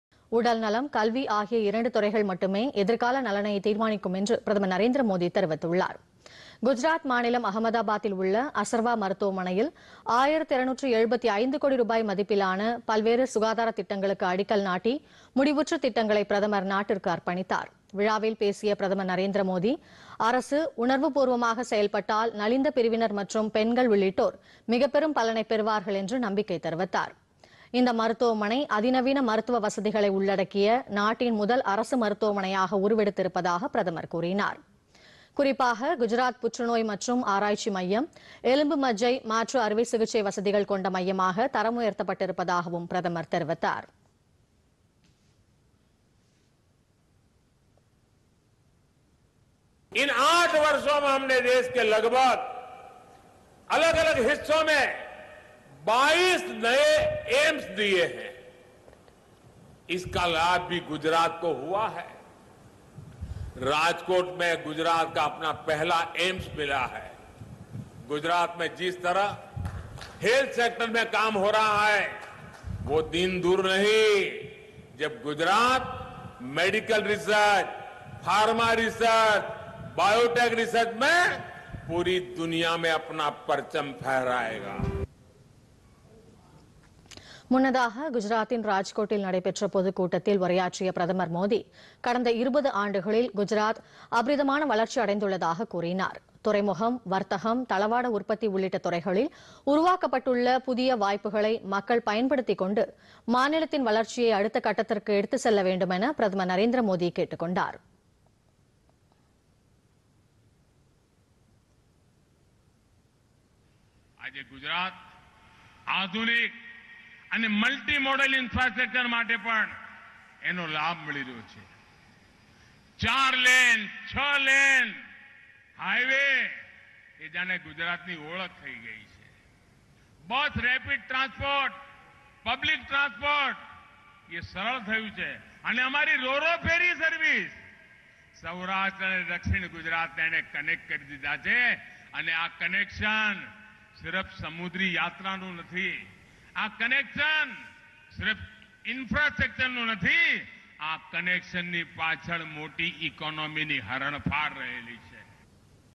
எதிர்கால நலனில் 2 துறைகள்PodhigaiTamilNews
உடல் நலம்,  கல்வி ஆகிய துறைகள் மட்டுமே எதிர்கால நலனை தீர்மானிக்கும் -  குஜராத்தில் ஆயிரத்து 275 கோடி ரூபாய் மதிப்பிலான திட்டங்களை தொடங்கி வைத்து பிரதமர் நரேந்திர மோதி பேச்சு.